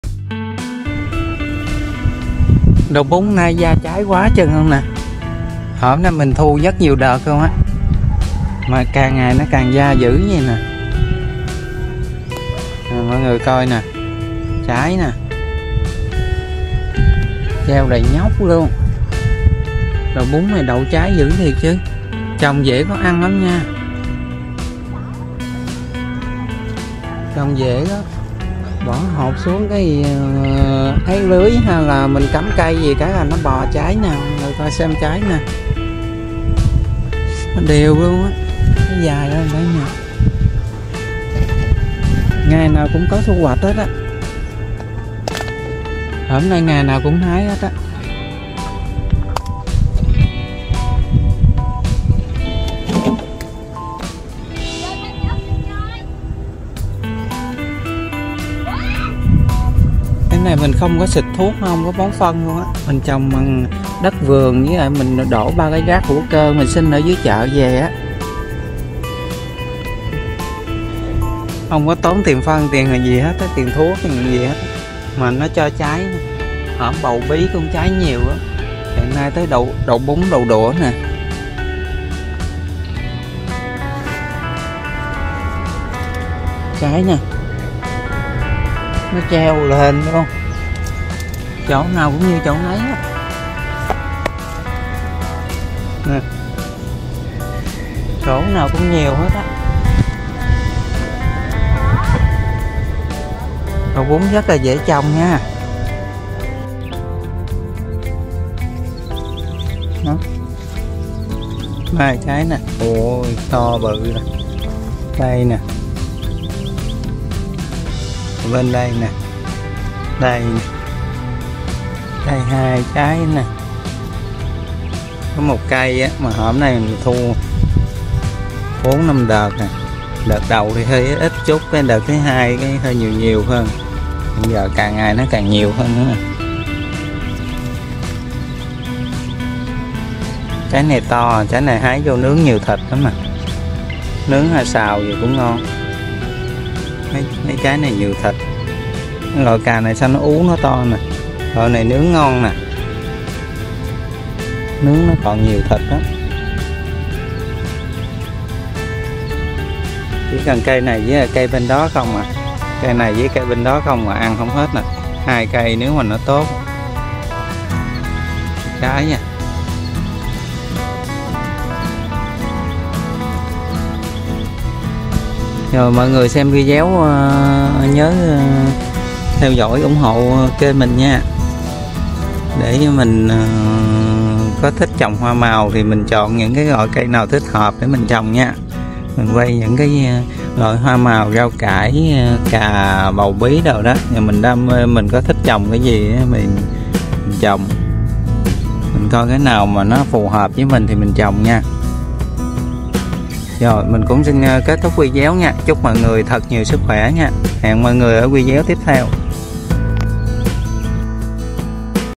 vườn rau hữu cơ ăn hoài không hết của ảo rất nhiều trái
Video mình chia sẻ lại vườn rau của ảo trồng hữu cơ rất nhiều trái ăn hoài không hết.thấy hữu ích đăng ký kênh ủng hộ mình nhé!cảm ơn mọi người rất nhiều!ảotrồngrau